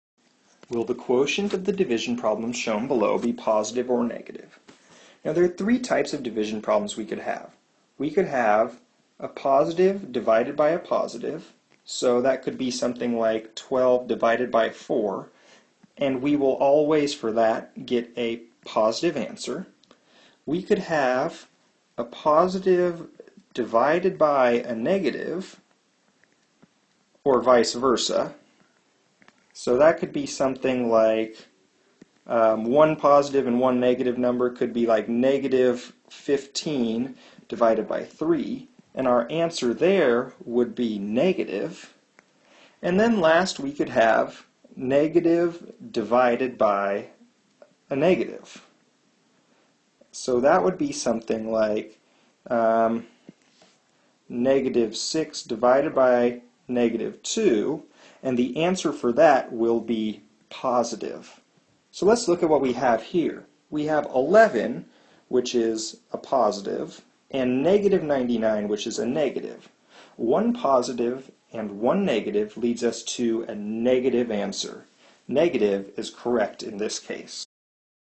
[7.NS.2b-1.1] Division of Signed Numbers - Common Core Standard
Students must know when dividing signed numbers, if the signs are the same, the result is positive and if the signs are different, the result is negative.